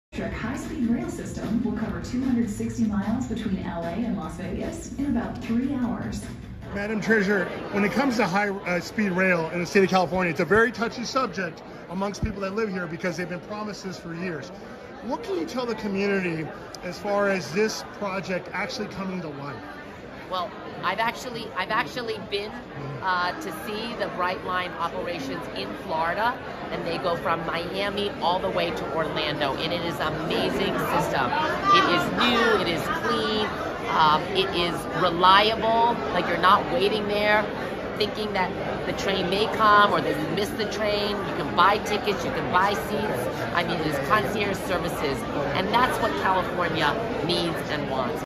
California State Treasurer Fiona Ma provided the latest update...
California State Treasurer Fiona Ma provided the latest update on the status of the Brightline West High-speed Rail project. High speed rail from the IE to Vegas.